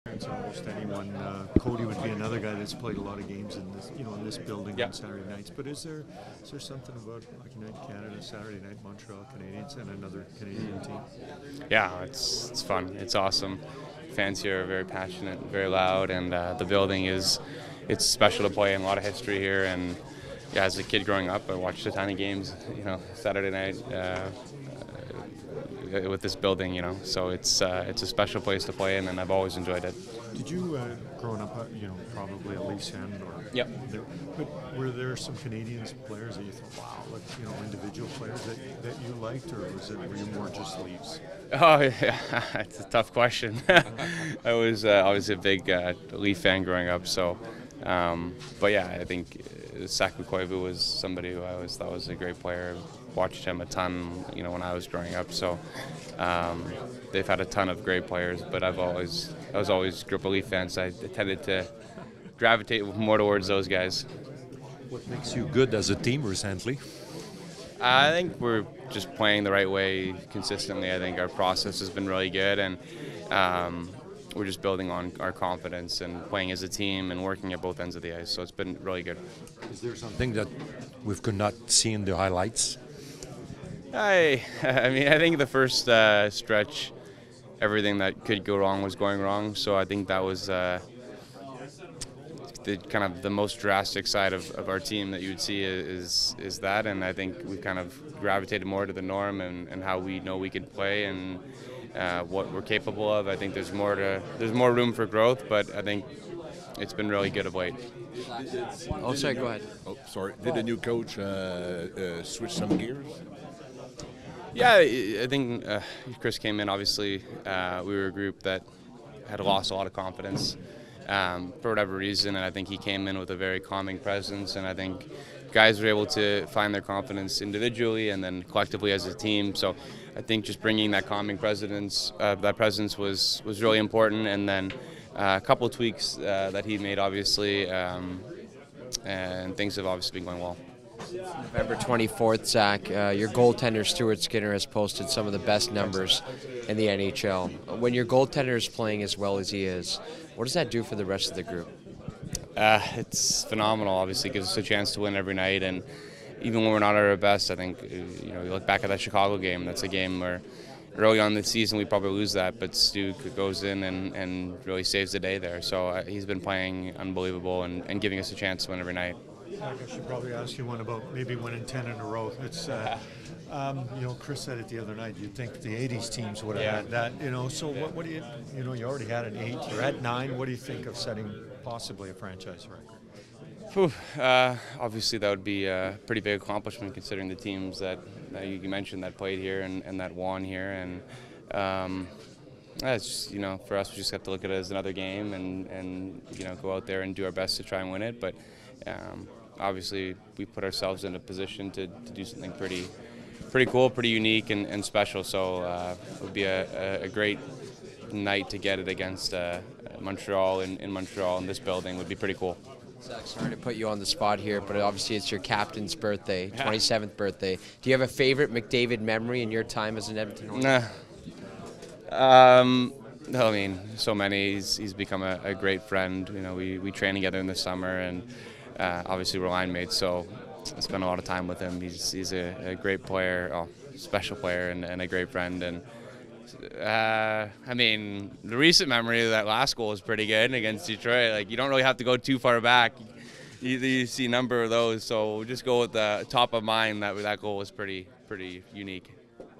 PRE-RAW | Zach Hyman 01.13.24
Zach speaks with the media as the team prepares for an electric atmosphere at the Bell Centre on Hockey Night in Canada.